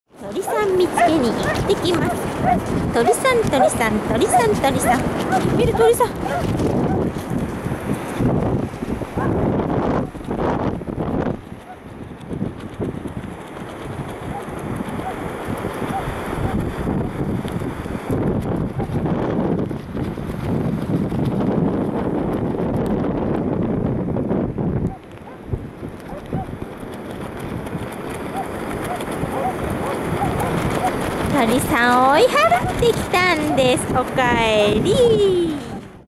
鳥さん!
台風がきてるけど、鳥さんの多いこと!!
だからミルが超ゴキゲンの旅になってます。

Marroppy's！